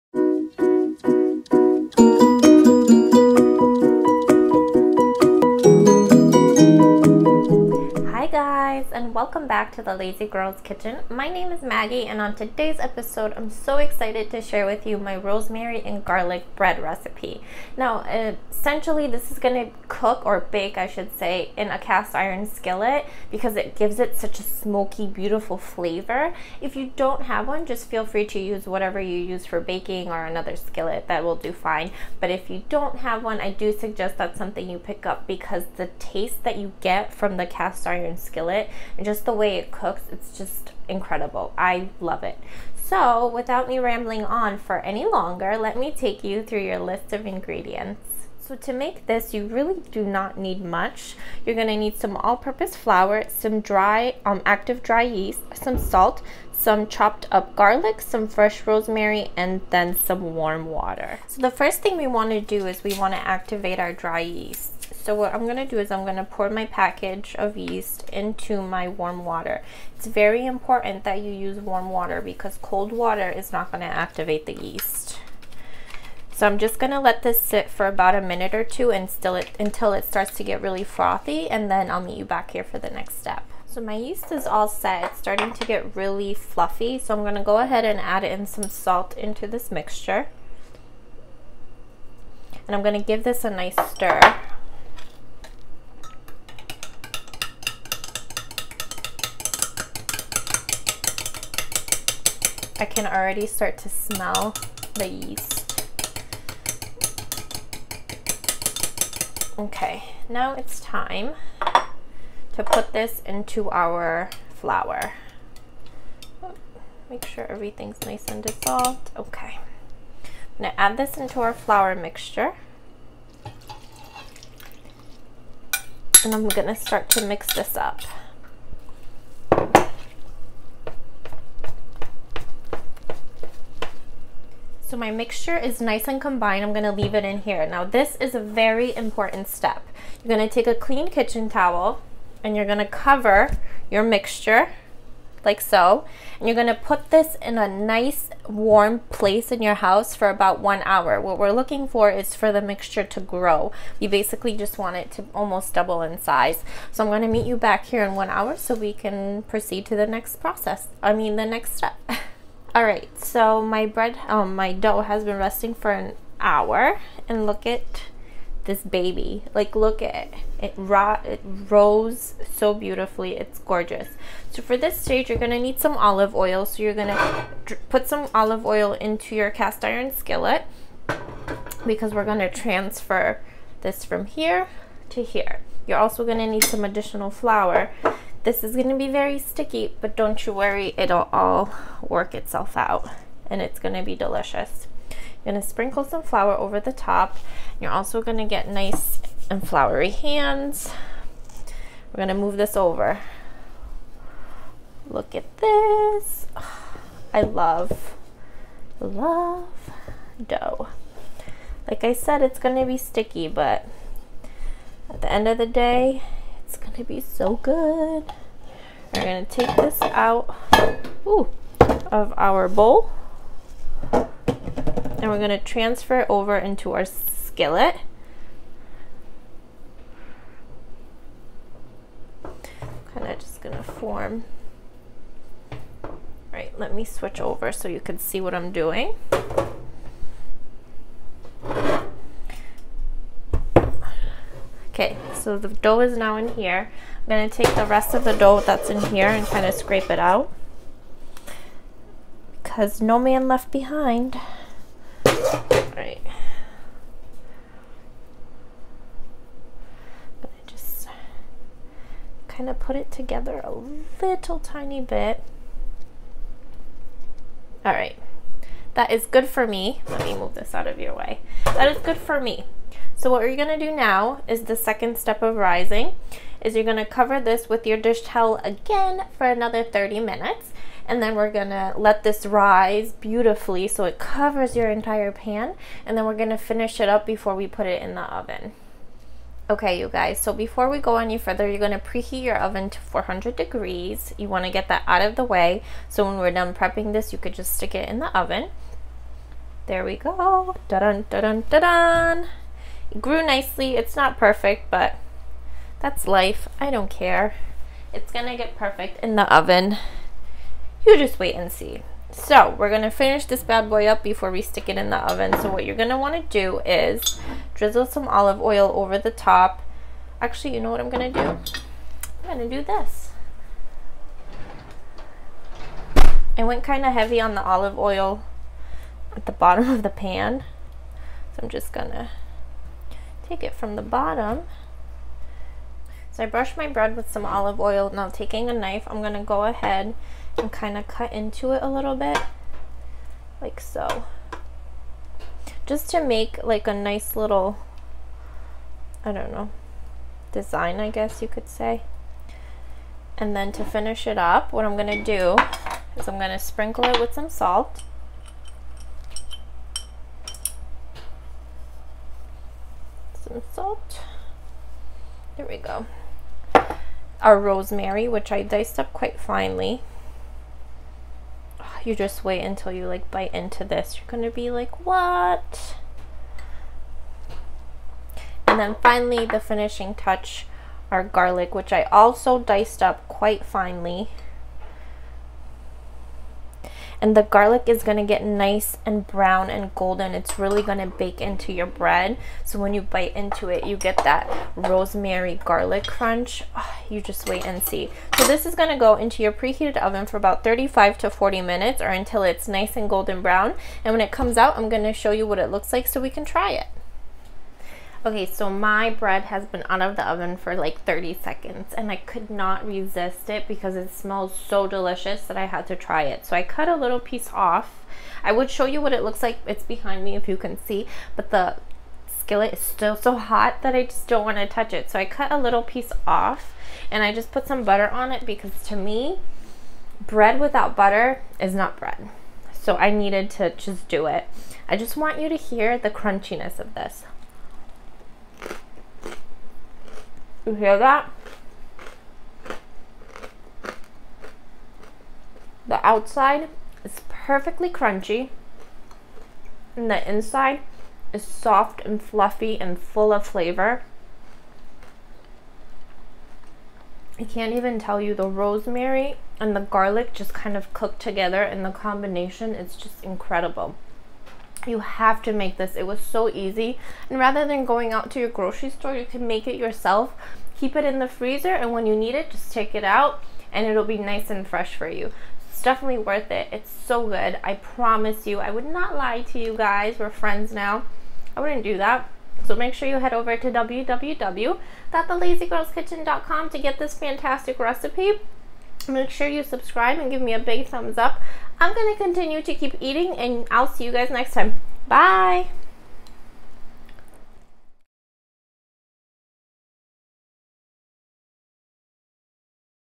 Rosemary and Garlic Bread
This homemade bread recipe is so easy and so delicious!